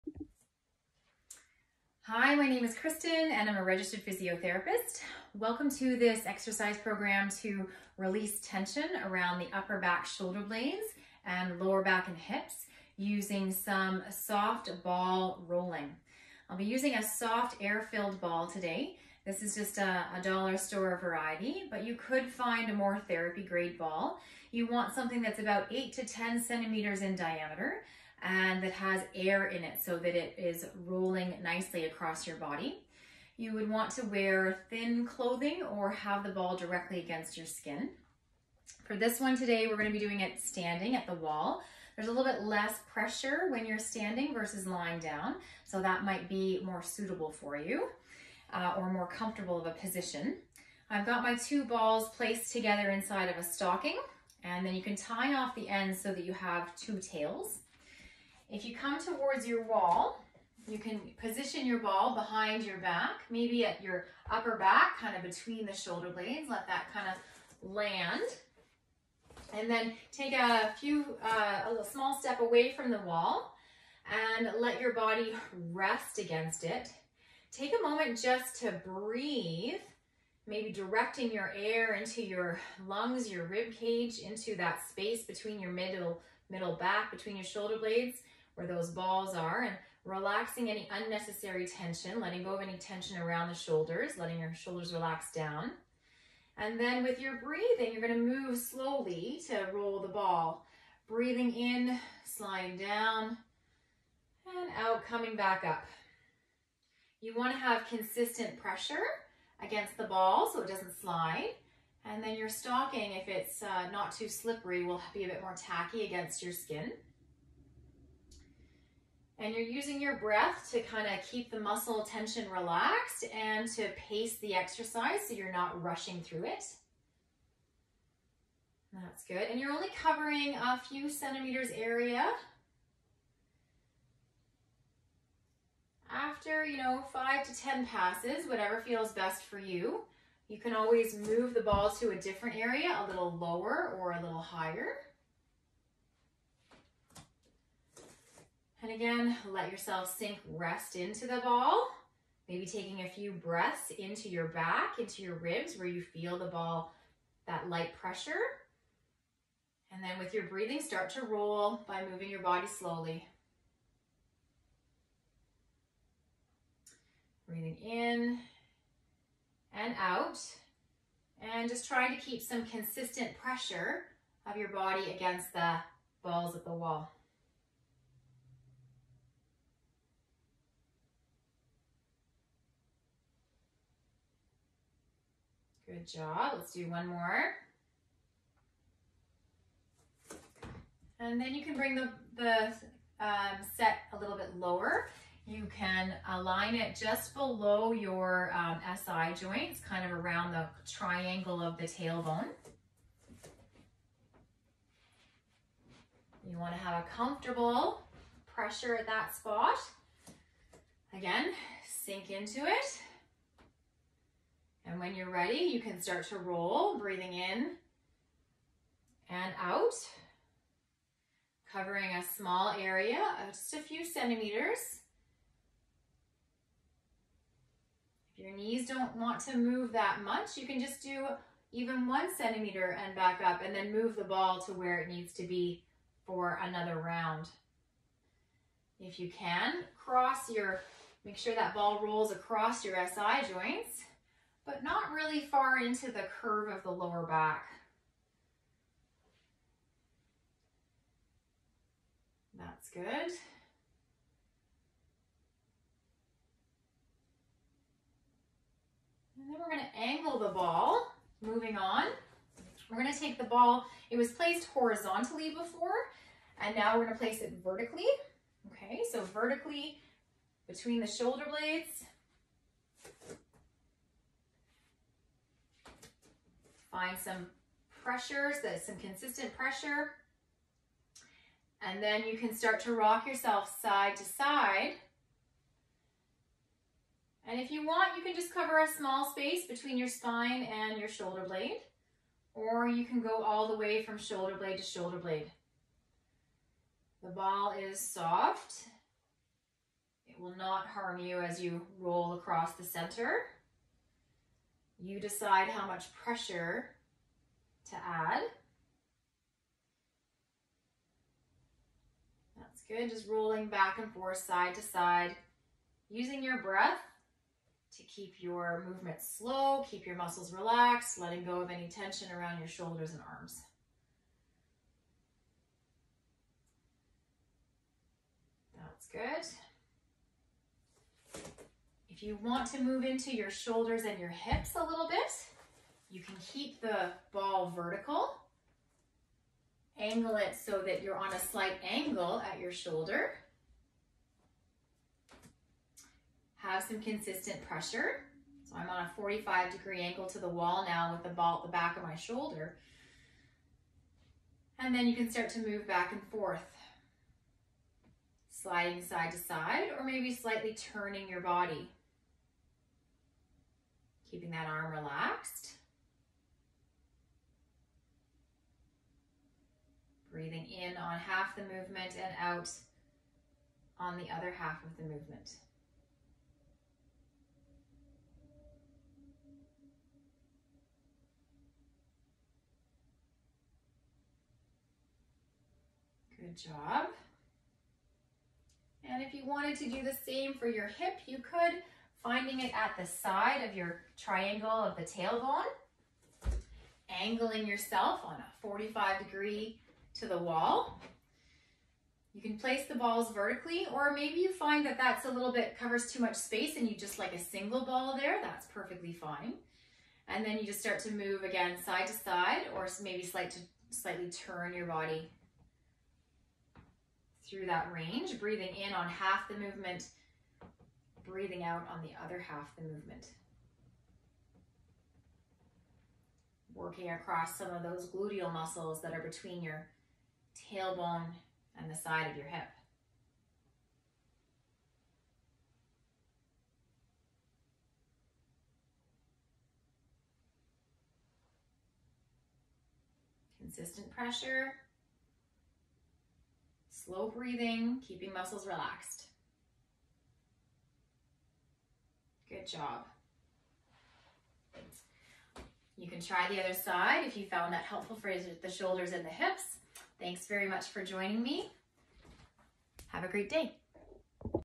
Muscle Release with Ball Rolling at the Wall
This movement practice uses soft air-filled ball to gently roll across and release muscle tension at upper and low back, hips and shoulders.